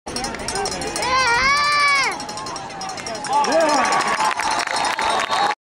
Grant Sherman Finds Kollin Stollar for 2!
2013 - Kenton Wildcat Football - Week 5

Kenton vs. Lima Shawnee @ Robinson Field

Kenton Wins 46-0!

Record 5-0 Overall and 4-0 in the Western Buckeye League!